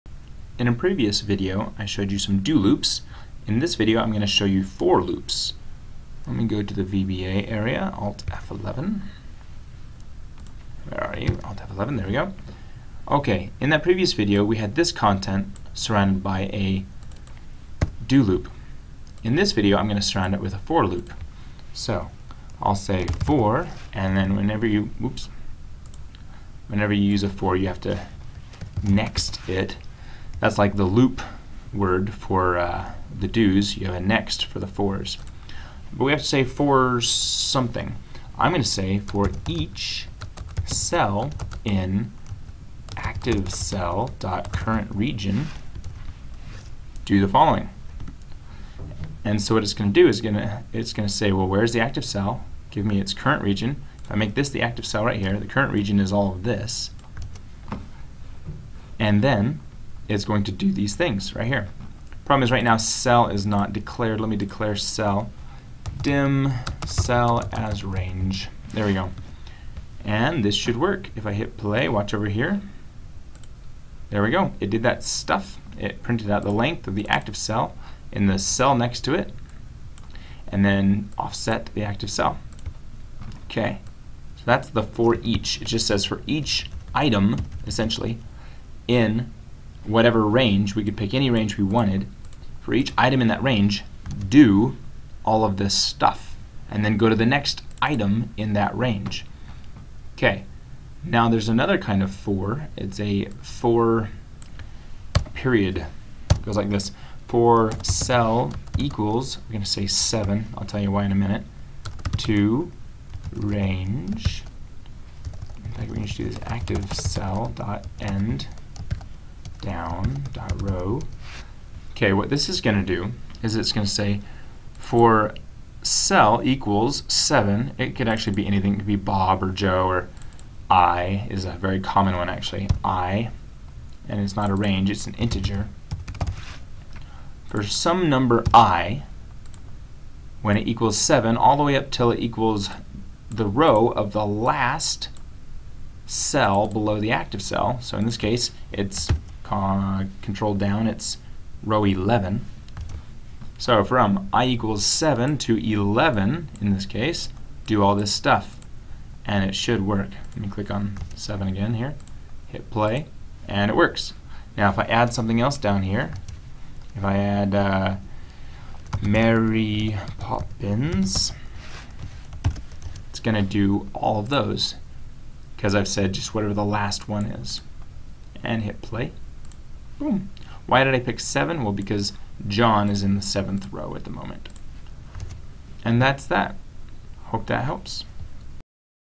Excel VBA For Loops
In this video I show how to use For and For Each loops.